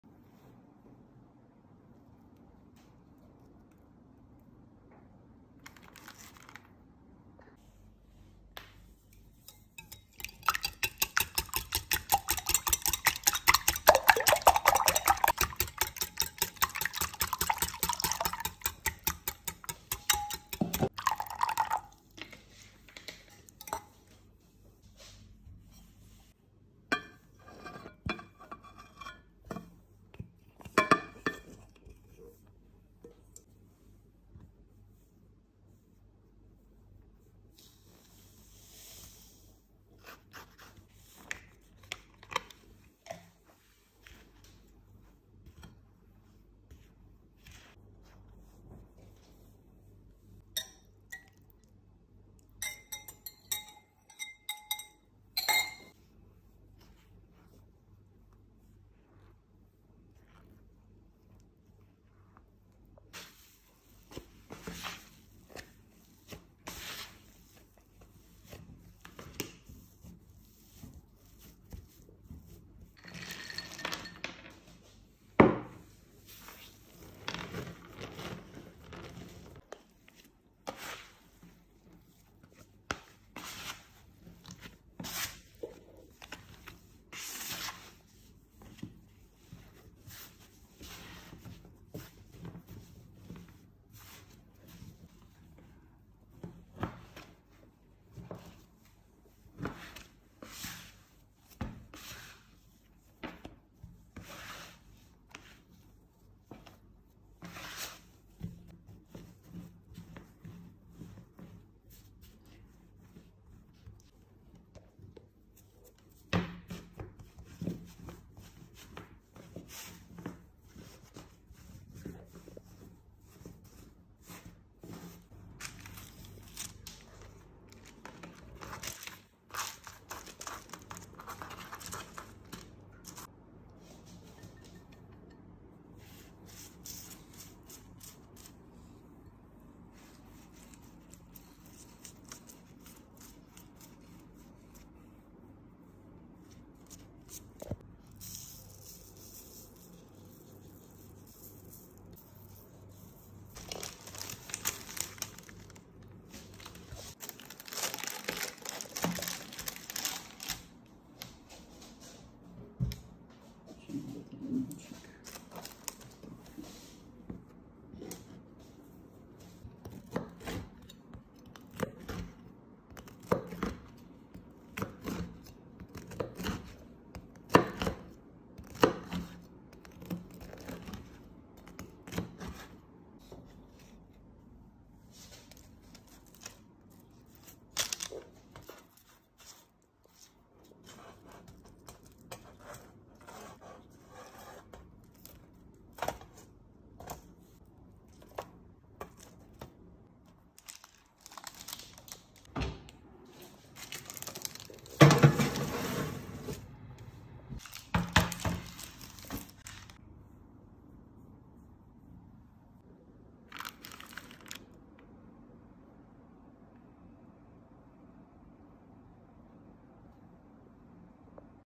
Grandma making original Cantucci- Almond Biscotti from Tuscany
Cantucci is a type of traditional Italian biscuit known as biscotti.
These very easy twice-baked almond cookies are extra crunchy, and usually paired with a sweet dessert wine such as vin santo or in some areas muscat.

Ingredients:
4 eggs
500 g all purpose flour
250 g sugar
200 g almonds
100 g unsalted butter
12 g baking powder
1 pinch of salt
Lemon zest of 1 lemon
1 tsp vanilla extract

Bake the logs of dough for 30 minutes at 180°C (350 °F).
Use a sharp knife to cut them at an angle into slices about ½-inch thick. Place the cookies on the pans, cut sides down, and bake for 10 minutes at 180 °C (350°F), or until golden.

Enjoy baking!